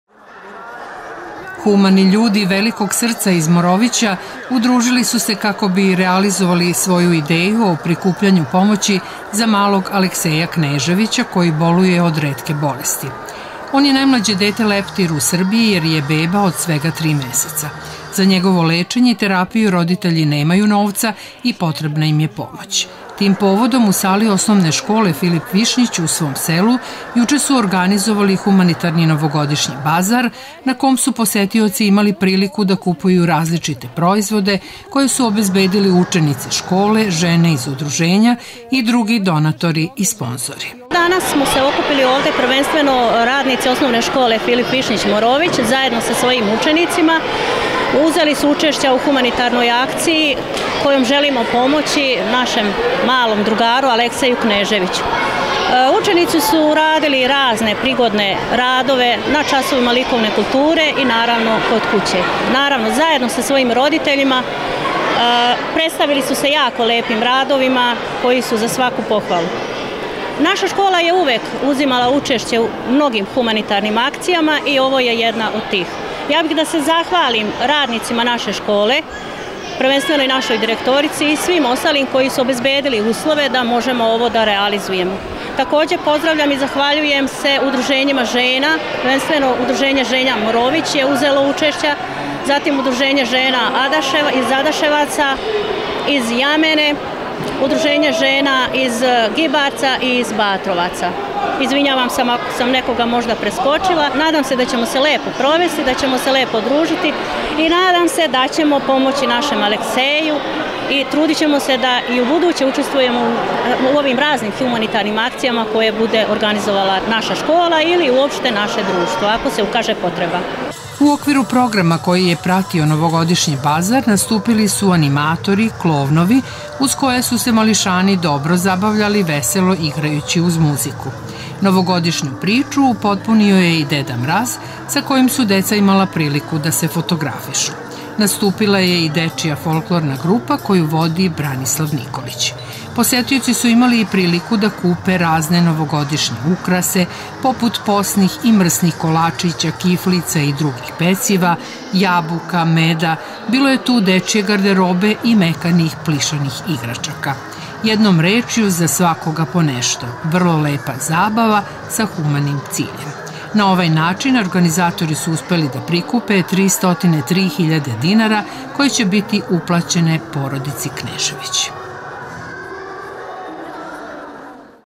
Humanitarni bazar u Moroviću
Humani ljudi  iz Morovića , udružili su se zajedno sa divnom idejom da pomognu porodici Knežević , za lečenje malog Alekseja koji boluje od retke bolesti. Juče su u sportskoj hali Osnovne škole „Filip Višnjić“ u svom selu organizovali  Novogodišnji humanitarni bazar kako bi prikupili sredstva . Na ovaj način uspeli su da prikupe 303.000 dinara.